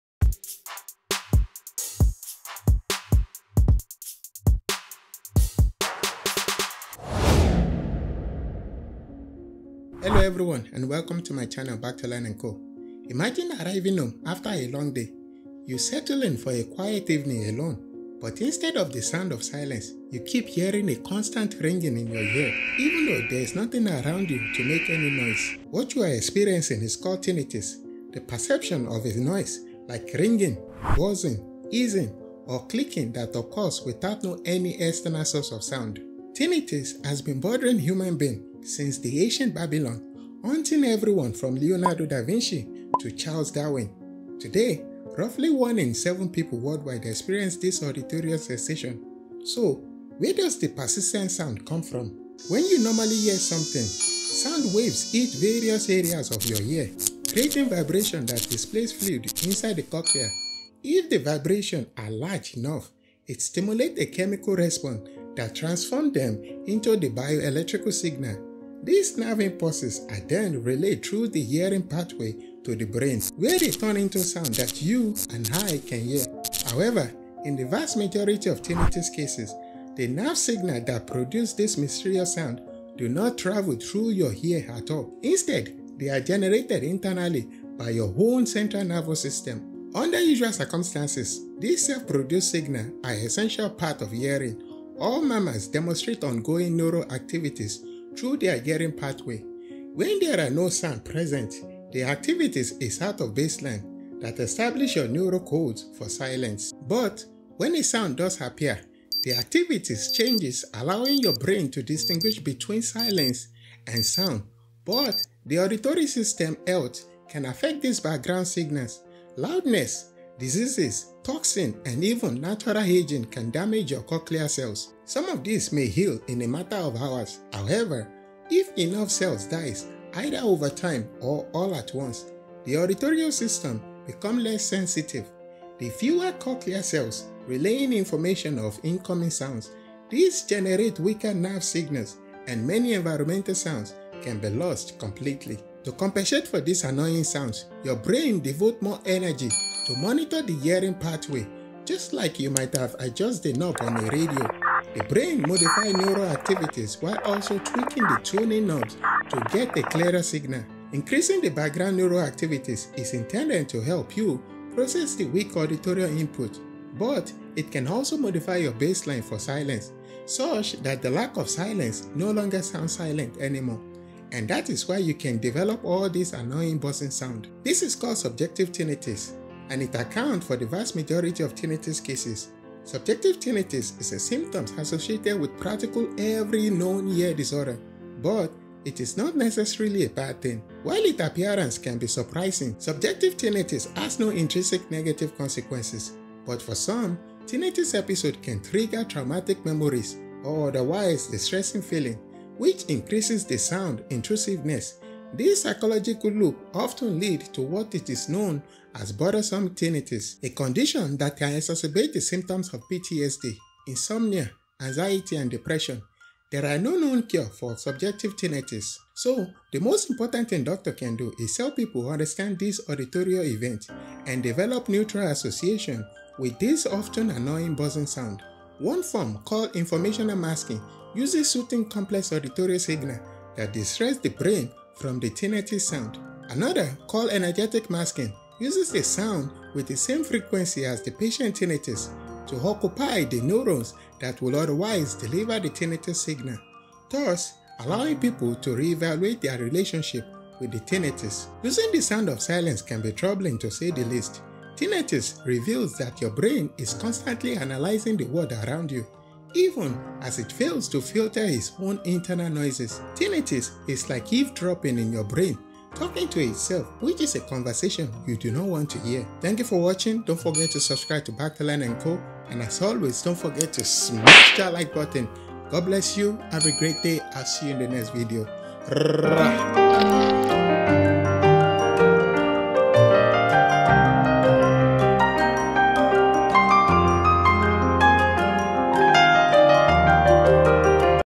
Annoying Constant Sounds In Your Ears
Imagine arriving home after
a long day, you
settle in for a quiet evening alone
 but instead of the sound of silence
 you hear a constant ringing in
yours hear, even though there’s
nothing making any noise.
what you are experiencing is
called tinnitus the perception of
a noise like ringing, buzzing,
hissing or clicking that occurs
without any external source
 of sound.
Tinnitus has been bothering
 human being
 since the ancient Barbelon, hunting
everyone from Leonardo da Vinci
to Charles Darwin.
Today roughly one in seven
people worldwide experiences
this auditory sensation, so where
 does this persistent sound come
 from? when you normally hear
 something sound waves hit
various areas of your ear creating
 vibrations that displaced fluid
inside the cochlea, if the
vibrations are large enough
it stimulates a chemical response
that transforms them into bio
electrical signals, this nerve
impulses are then relayed through
the hearing pathway to the brain,
 where they turn into the sounds
we can hear. However in the vast
 majority of tinnitus cases, the
 nerve signals that produce these
 mysterious sounds do not travel
through your ear at all. instead
they are generated internally by your
 own central nervous system. under
 usual circumstances these
self-produced signals are an
an essential part of the hearing, all
mammals demonstrate ongoing
neural activity through their
earring pathways. when there
are no sounds present this
activity is out of baseline that
establishes your neural
code for silence.
when a sound does appear this
 activity changes allowing the
 brain to distinguish between
silence and sound but the auditory
 system health can affect this
 background signals, loud noises,
diseases, toxins, and even natural
aging can damage your cochlear
cells, some of this may heal in a
 matter of hours, however if enough
 cells die either overtime or
all at once, the auditory system
 become less sensitive, with
fewer cochlear cells relaying
 information of incoming sounds
this generate wicker nerve signals
and many environmental sounds
can be lost completely. To compensate
 your brain devotes more energy
to monitoring the hearing pathway
 just like you might adjust the
knobs of a radio.
the brain modifies the neural activity
 while also tweaking the tuning
 knob to get a clearer signal.
Increasing this background Neurol
 activity is intended to help you
 process weak auditory inputs,
but it can also modify your baseline
 for silence such that the lack
 of sound no longer sounds
silent any more, and that is
why you can develop the
annoying buzzing sound.
this is called subjective tinnitus
and it accounts for the vast
majority of tinnitus cases,
subjective tinnitus is a symptom
 associated with practically every
 known ear disorder but it is not
necessarily a bad thing,
while its appearance can be
surprising, subjective tinnitus
has no intrinsic negative
consequences but for some, tinnitus
 episodes can trigger traumatic
memories or otherwise distressing
 feelings which increase the
sounds intrusiveness this
psychological loop often leads
to what is known as bothersome
tinnitus, a condition that can
exacerbate the symptoms of PTSD,
insomnia, anxiety, and depression
there is no known cure for subjective
 tinnitus. so the most important thing
 doctors can do is help people
understand this auditory event and
 develop neutral associations with
 these often annoying buzzing sounds.
For example, sound therapy uses
noises like rain, bird song or
music to mask tinnitus sound
 which can reduce stress.
one form called informational
masking uses soothing complex
 auditory signals that distract
 the brain from the tinnitus
sound, another called energetic
 masking uses sounds with the
 same frequency as the patient’s
 tinnitus to occupy the neurons
 that would otherwise deliver
the tinnitus signal, thus
allowing people to
 reevaluate their relationship
with tinnitus.
losing the sound of silence
 can be troubling to say
the least, tinnitus reveals that
your brain is constantly analyzing
 the world around you, even
as it fails to filter it's own internal
 noises.
 Tinnitus is like eavesdropping on
your brain talking to itself
which is a conversation you
 don’t want to hear.

References:
TedED

YouTube, Facebook, Twitter, and IG: @Backtolandandco